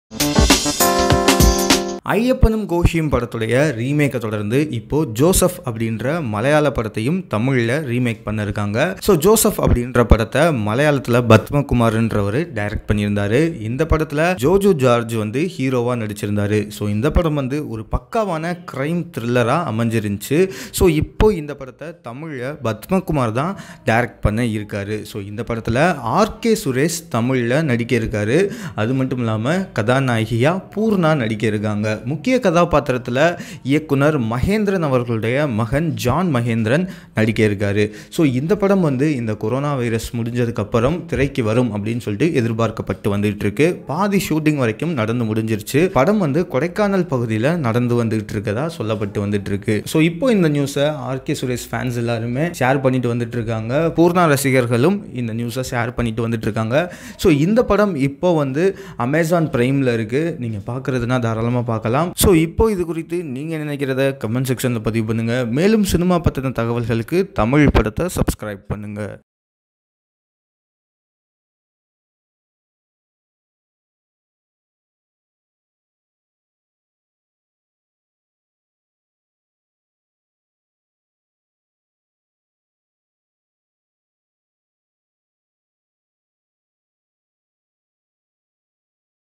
RK Suresh to play a double role in the Tamil remake of Joseph | Latest Tamil Movie Updates
RK Suresh to play a double role in the Tamil remake of Joseph. Actor RK Suresh will be playing the protagonist in one of the successful and widely acclaimed Malayalam films of 2018, Joseph.